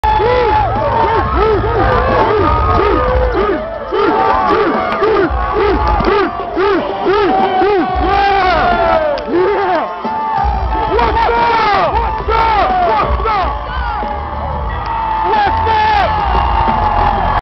R-truth exiting the ring birmingham live wwe lg arena
Wwe raw live birmingham england Lg arena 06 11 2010